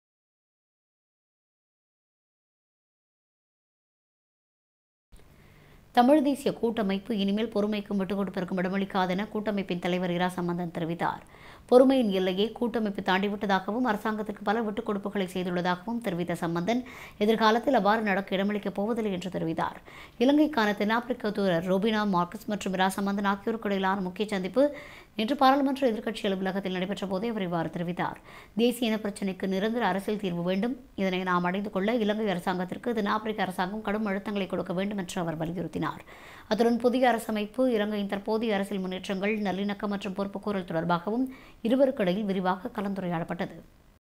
தமிழ் தேசிய கூட்டமைப்பு இனிமேல் பொறுமைக்கும் விட்டுக்கொடுப்பிற்கும் இடமளிக்காது - இரா.சம்பந்தன்
தமிழ் தேசிய கூட்டமைப்பு இனிமேல் பொறுமைக்கும் விட்டுக்கொடுப்பிற்கும் இடமளிக்காது என  கூட்டமைப்பின் தலைவர் இரா.சம்பந்தன் தெரிவித்துள்ளார். பொறுமையின் எல்லையை கூட்டமைப்பு தாண்டிவிட்டதாகவும், அரசாங்கத்திற்கு பல விட்டுக்கொடுப்புக்களை  செய்துள்ளதாகவும் தெரிவித்த சம்பந்தன், எதிர்காலத்தில் அவ்வாறு நடக்க இடமளிக்கம போவதில்லையென தெரிவித்துள்ளார். இலங்கைக்கான தென்னாபிரிக்க தூதுவர் ரொபினா மார்க்ஸ் மற்றும்  இரா.சம்பந்தன் ஆகியோருக்கிடையிலான முக்கிய சந்திப்பு நேற்று பாராளுமன்ற எதிர்க்கட்சி அலுவலகத்தில் நடைபெற்ற போதே அவர் மேற்கண்டவாறு தெரிவித்தார். தேசிய இனப்பிரச்சினைக்கு நிரந்தர அரசியல் தீர்வு வேண்டும். இதனை நாம் அடைந்து கொள்ள இலங்கை அரசாங்கத்திற்கு தென்னாபிரிக்க அரசாங்கம் கடும் அழுத்தங்களைக் கொடுக்க வேண்டும் என அவர் வலியுறுத்தினார்.  அத்துடன் புதிய அரசமைப்பு, இலங்கையின் தற்போதைய அரசியல் முன்னேற்றங்கள், நல்லிணக்கம் மற்றும் பொறுப்புக்கூறல் தொடர்பாகவும் இருவருக்குமிடையில் விரிவாக கலந்துரையாடப்பட்டுள்ளது.